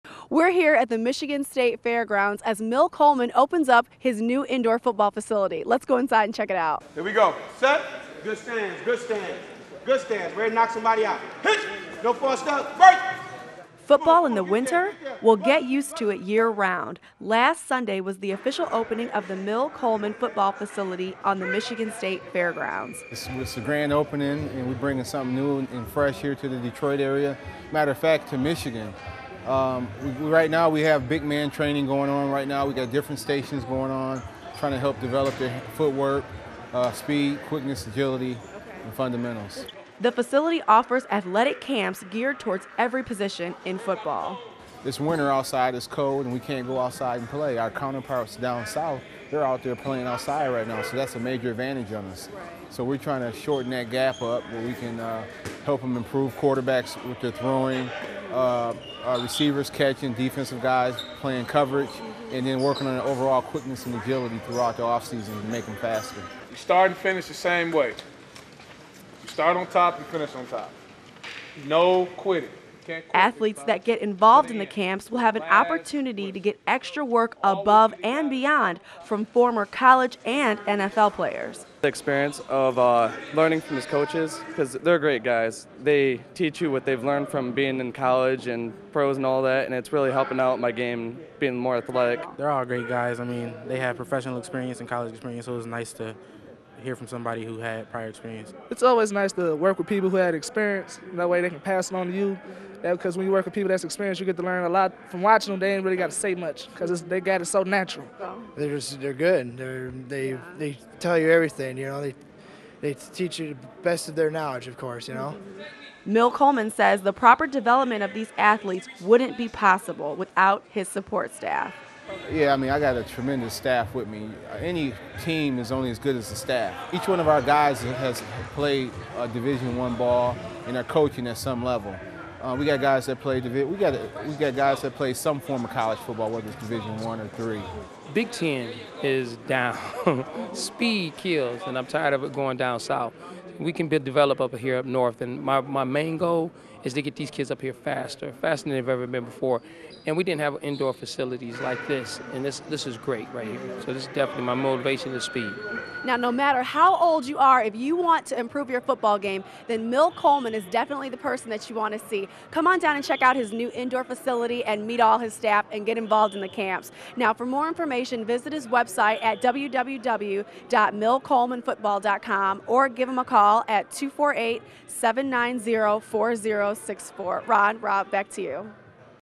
Mill Coleman Football Facility
Let's meet Mill Coleman and his football facility on the Michigan State Fairgrounds